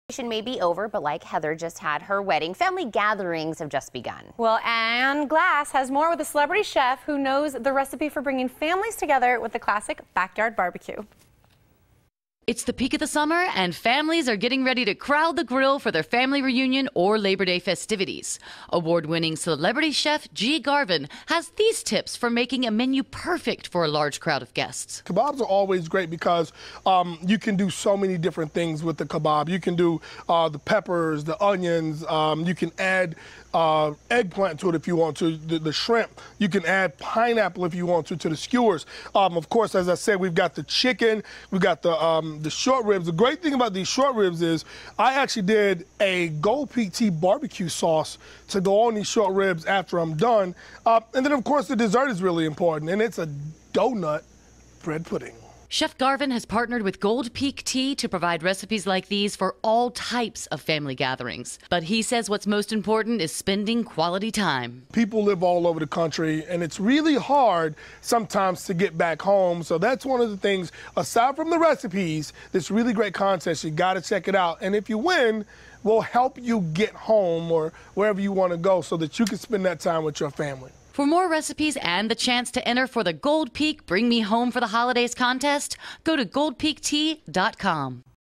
Chef Garvin Summer BBQ
Summer vacation may be over, but family gatherings have just begun! Ann Glass has more with a celebrity chef who knows the recipe for bringing families together with the classic backyard barbecue.